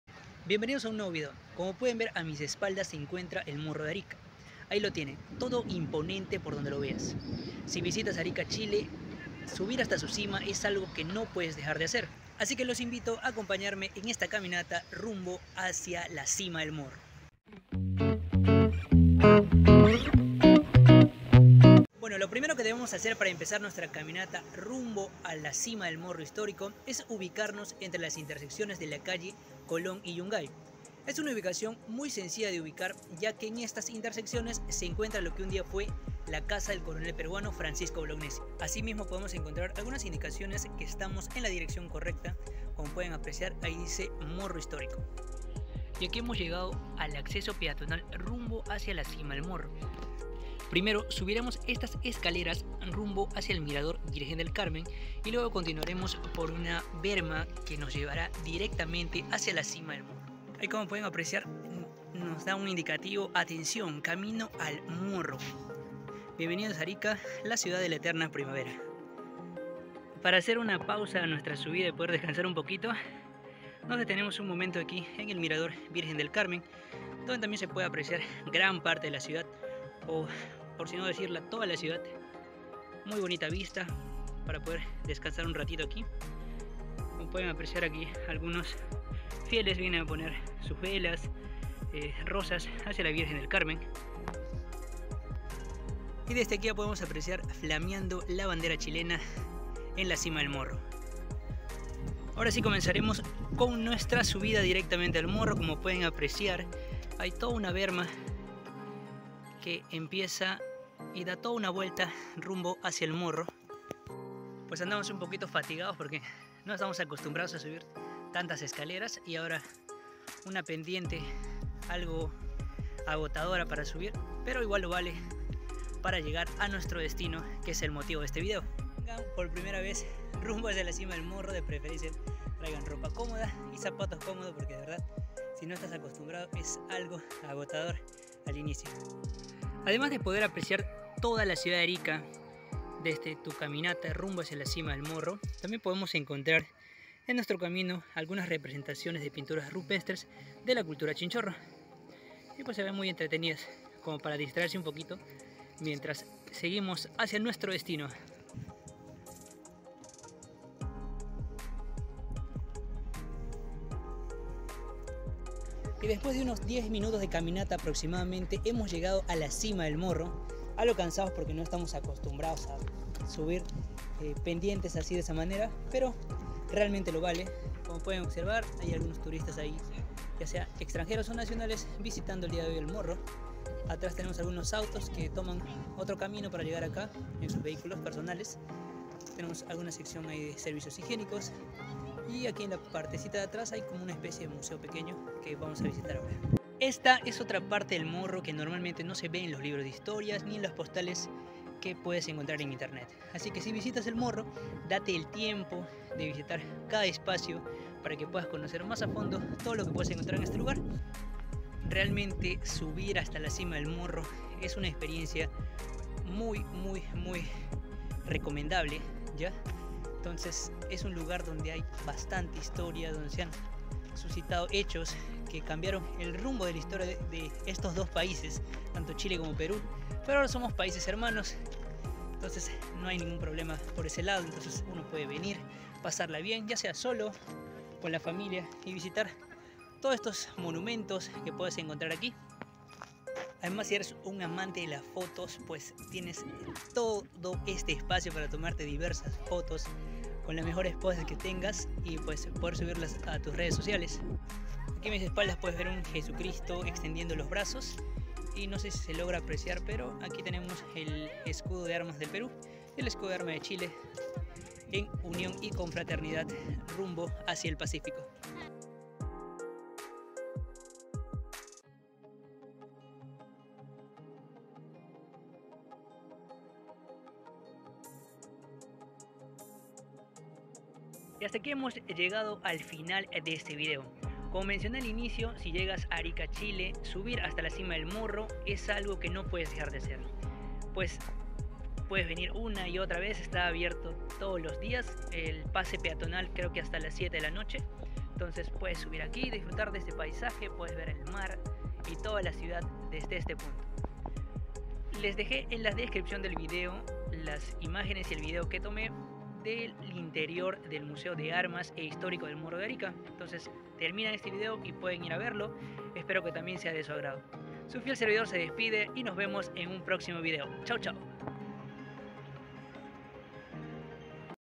"Morro de Arica" » La fortaleza más importante de Chile 🇨🇱 «
Subímos hasta la cima del morro histórico de Arica 🇨🇱

Fotos del interior del Museo Histórico y de armas del Morro

Este vídeo fue editado con YouCut - Editor de videos Échale un vistazo y descárgatela

La fortaleza más importante de Chile
Morro de Arica
Arica siempre Arica